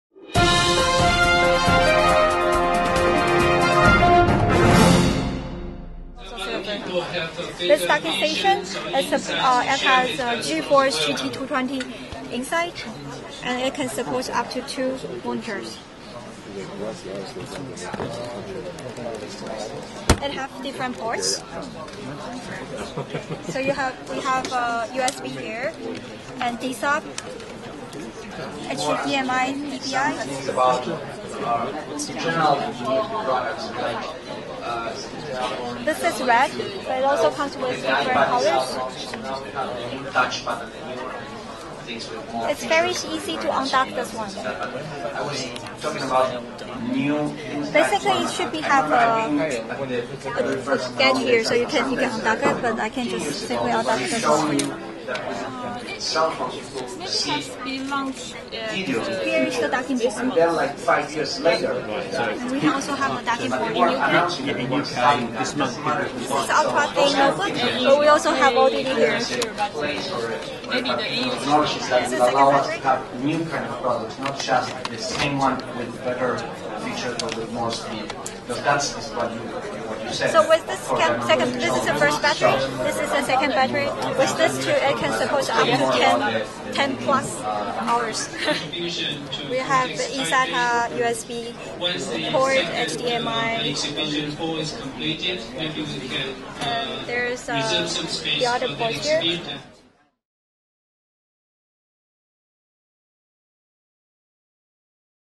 COMPUTEX 2010 - GIGABYTE Unveils Docking Ultra Think M1405 Notebook
- GIGABYTE unveiled an interesting docking notebook. The notebook itself is less than an inch thick and weighs 1.6kg. Sporting a Core 2 Duo ULV or Celeron ULV processor, the notebook supports an optical drive or a battery that increases runtime to 10+ hrs. The notebook has tons of expansion ports already, but the dock takes it up a notch by offering an upgraded GPU to an nVidia GT220 from the built in GMA4500, and allows support for up to 3 monitors. This will allow GIGABYTE to build on this platform by allowing support for more powerful GPUs in the future, giving your notebook desktop like performance when docked at home. This is enhanced by the overclocking capabilities and its always on USB charging capability for cellphones and other gadgets.